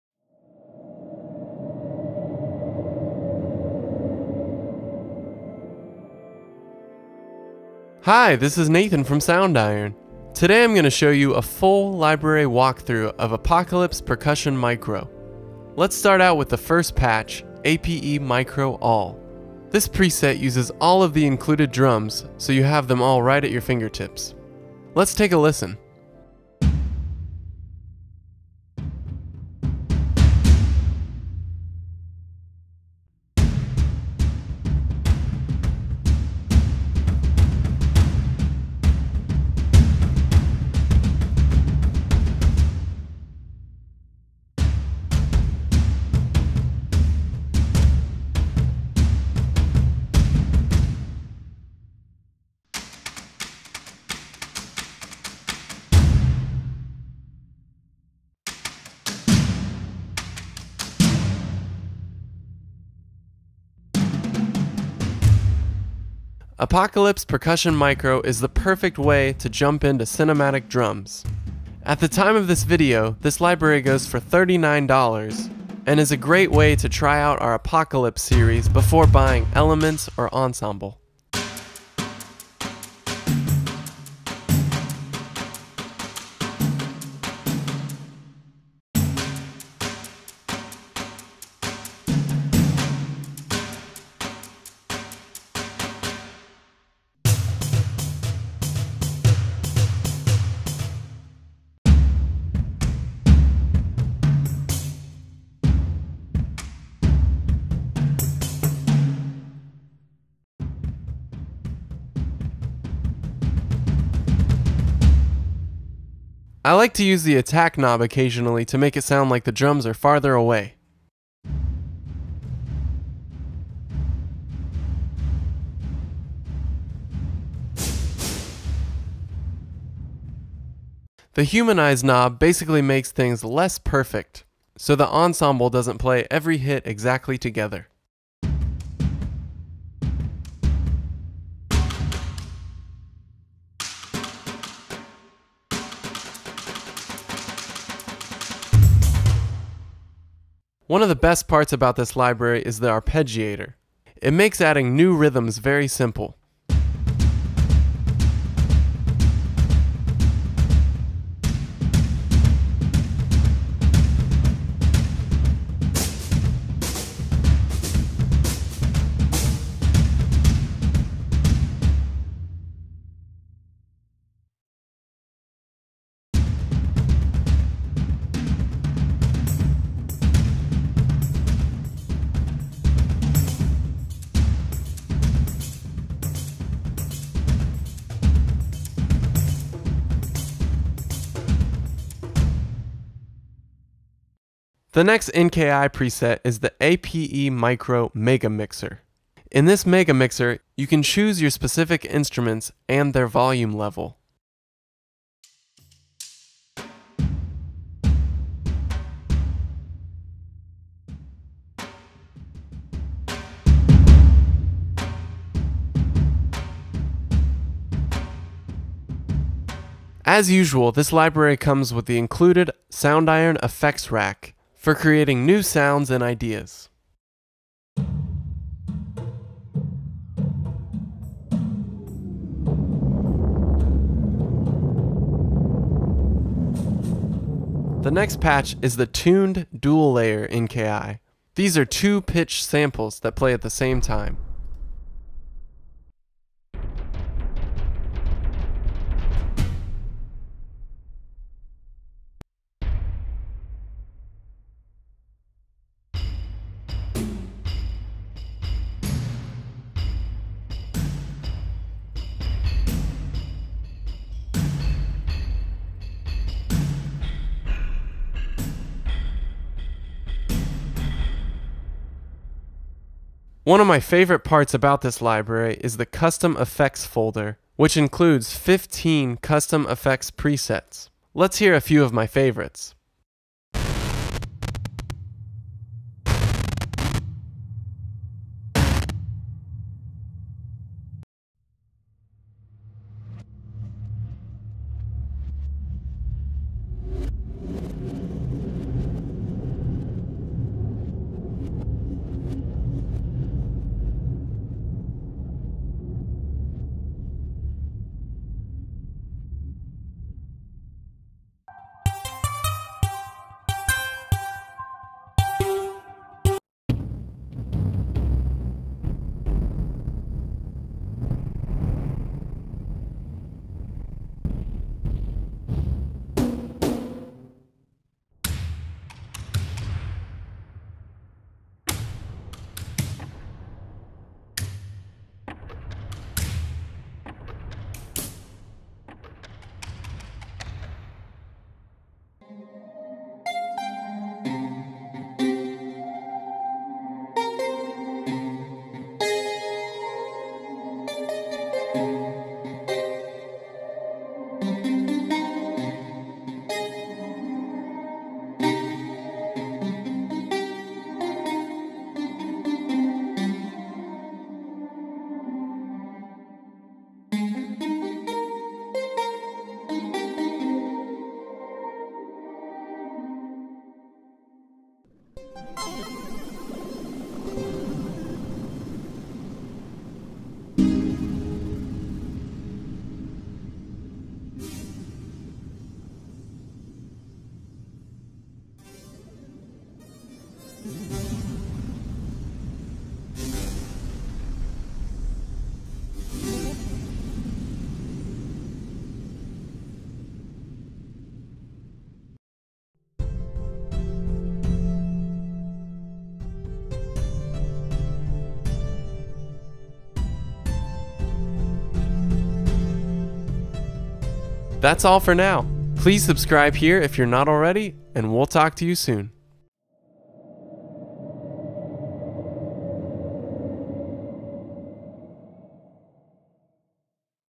Soundiron - Apocalypse Percussion Micro | Full Walk-Through
In this video Nathan Boler walks you through Apocalypse Percussion Micro showcasing the different patches of the library.

Apocalypse Percussion Micro is the compact starter edition of our acclaimed Apocalypse Percussion Ensemble library. This heavy cinematic drum library features a total of 28 hard-hitting ensemble articulations, including 6 concert snare drum sets, 5 large tom sets, 7 bass drum sets, 4 Indian Dhol sets, 3 Turkish Doumbek sets, 1 Irish Bodhrán (frame drum) articulation, one Egyptian Riq and a Greek Daf articulation, so you have access to a broad range of powerful sounds to create with. Apocalypse Percussion Micro has a simple and intuitive user interface with full articulation control, modular kit-building, dynamic arpeggiation and customizable FX routing, with 119 real-world and special effect convolution spaces.

Each articulation we performed by an ensemble of each drum type, ranging in size between 3 and 6 players in unison, for a massively epic sound. Each articulation sound set has 4 - 12 dynamic velocity layers and 2 round robin variations per note. We captured these instrument ensembles in the same bright, open hall that we recorded our legendary Olympus Symphonic Choir, Mercury Boy’s Choir, Montclarion Grand Piano, Struck Grand, Tuned Artillery and many other Soundiron libraries. the space and wide large-diaphragm close mic array give these drums a potent and aggressively open sound, with just the right touch of transparent natural reverberation. This library also includes 15 custom sound-designed FX presets to expand your create options.